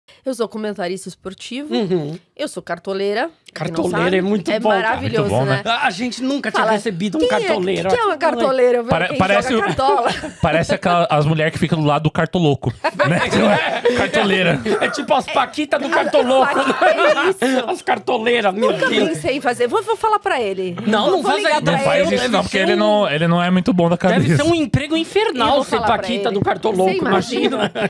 IMAGINA QUE INFERNO É SER PAQUITA DO CARTOLOUCO?! | Carol Bernardi
Carol Bernardi é Cartoleira e comentarista esportiva ligada ao time do Santos e ao canal De Olho no Peixe.